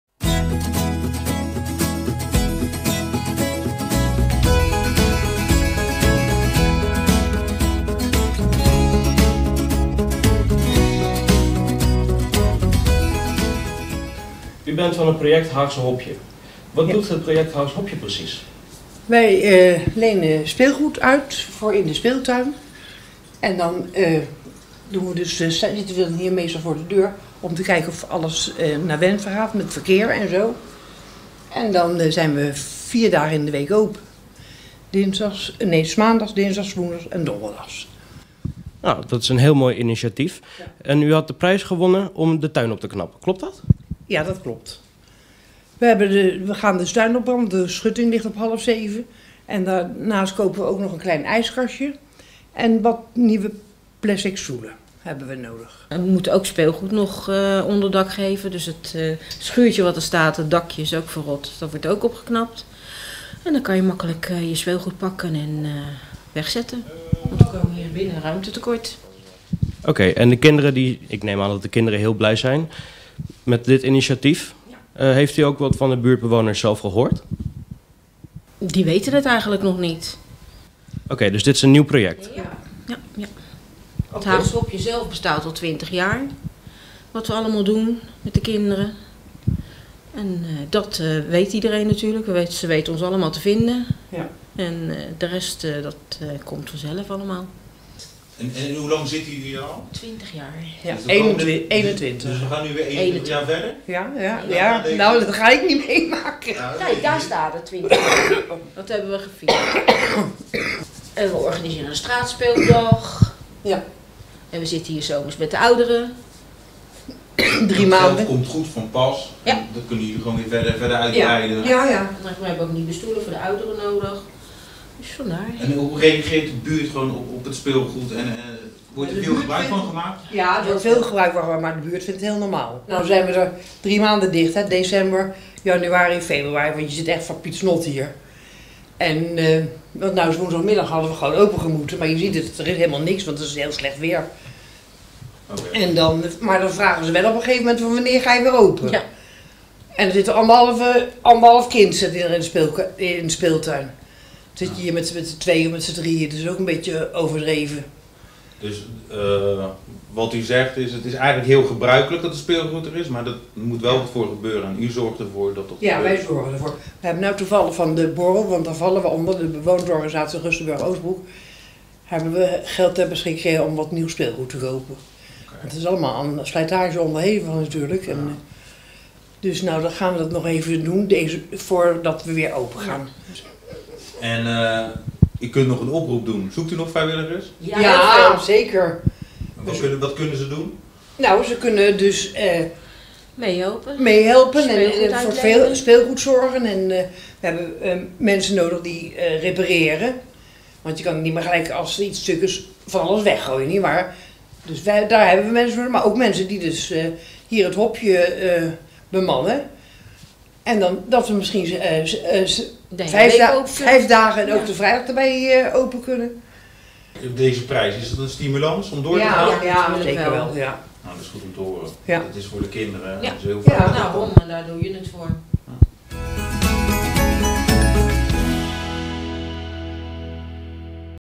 RTV Discus Haags Hopje Prijswinnaar ‘Mijn Thuis’
Speelgoeduitleen Haags Hopje aan de Gooilaan heeft een prijs gewonnen met de prijsvraag ‘Mijn Thuis’ van Staedion om de tuin op te knappen.